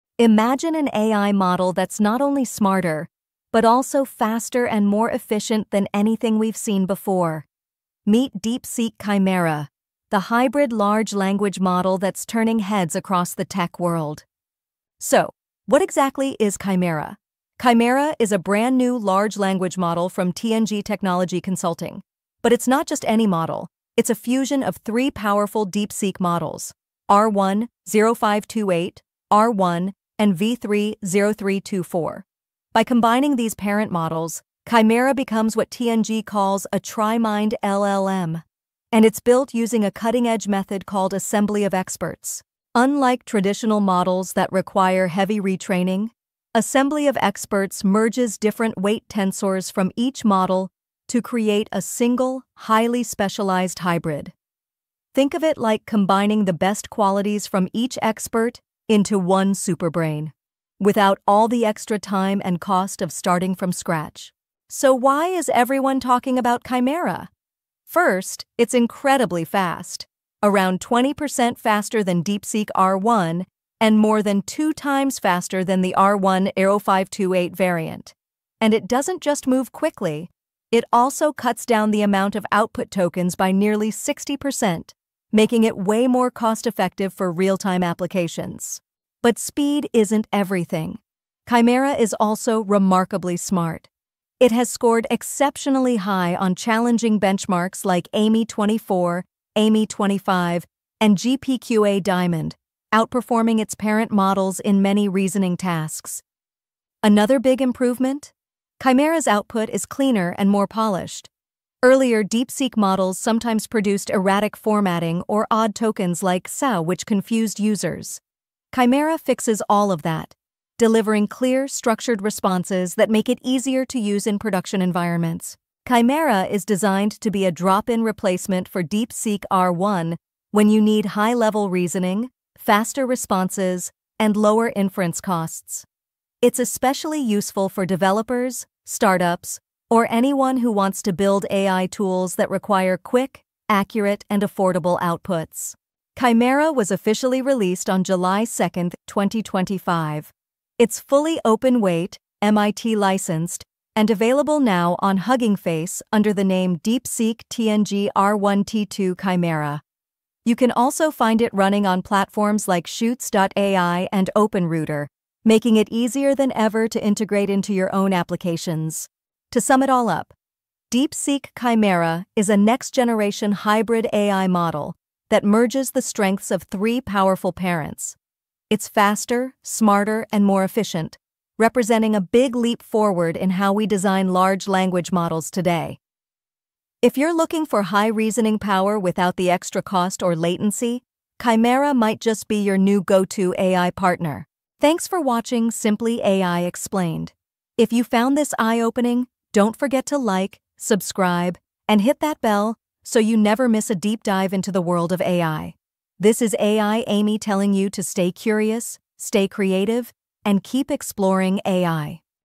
DeepSeek Chimera — The New Hybrid AI Model Shocking the Industry
A brand-new AI model has just arrived, and it's turning heads everywhere. Introducing DeepSeek Chimera — a revolutionary hybrid model that combines the best of multiple AI architectures to push the boundaries of what’s possible.

In this video, we break down exactly what makes Chimera so unique, how it merges different model approaches, and what this means for the future of artificial intelligence. From advanced reasoning capabilities to faster, more accurate outputs, Chimera is set to change the game for researchers, developers, and everyday users.

✨ What you’ll learn:

What is DeepSeek Chimera?

How does it work and what makes it a "hybrid" model?

How it compares to other leading models like GPT-4 and Gemini

Why this could be a major leap forward for AI applications

Whether you’re an AI enthusiast, a tech professional, or just curious about the latest breakthroughs, this video will give you all the insights you need.